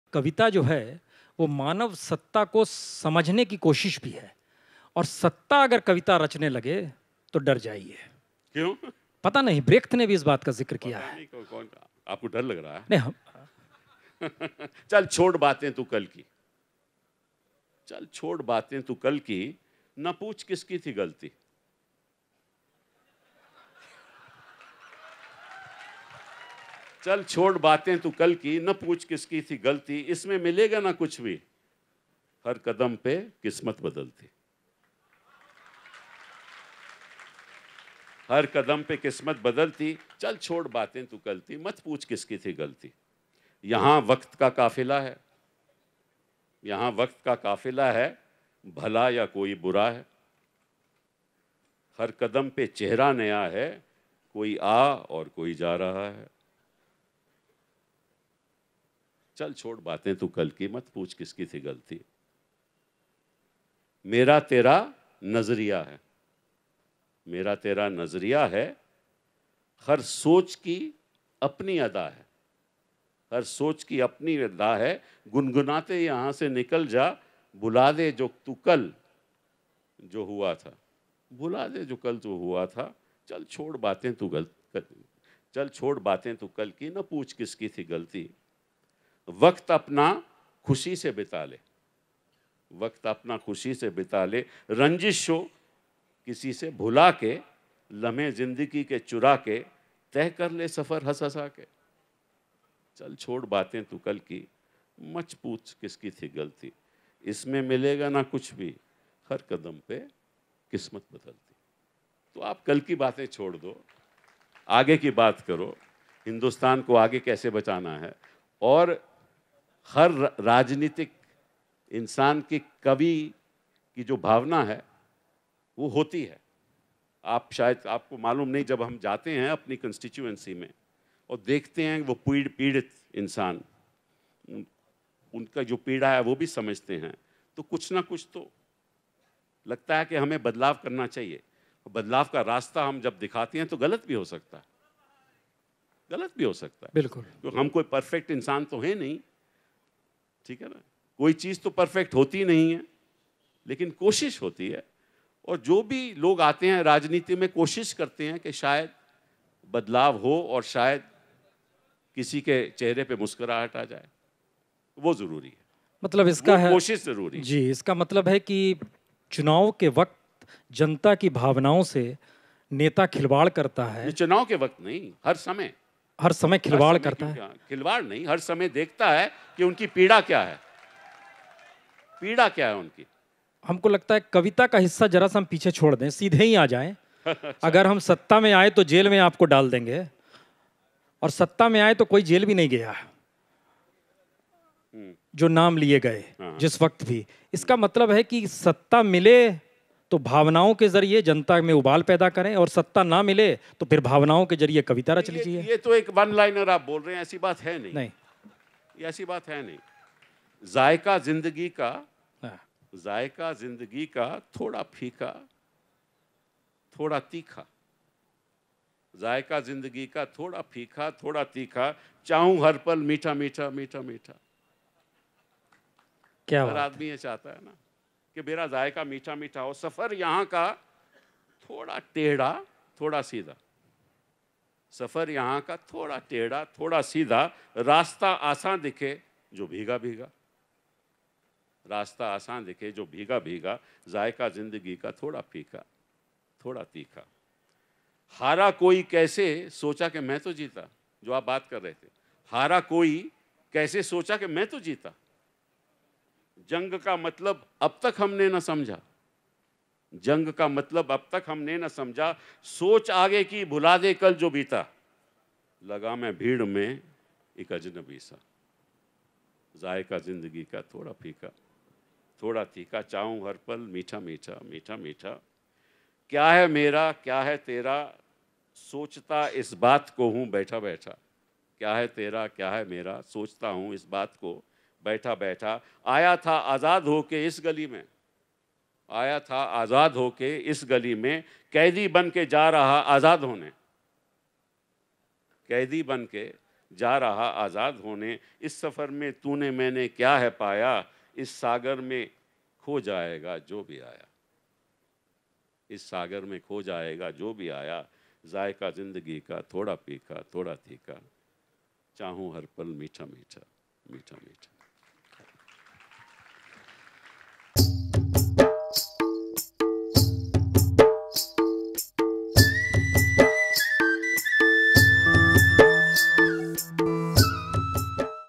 Corona के दौर में Kapil Sibal की कविता 'हर कदम पे किस्मत बदलती' | Sahitya Tak
चल छोड़ बातें तू कल की,
न पूछ किसकी थी गलती.
इसमें मिलेगा न कुछ भी,
हर कदम पे किस्मत बदलती...मशहूर वकील और केंद्र में मंत्री रहे कपिल सिब्बल ने साहित्य आजतक के मंच पर किस्मत पर लिखी एक कविता पढ़ी.कोरोना काल के इस समय में यह कविता बहुत सटीक बैठती है.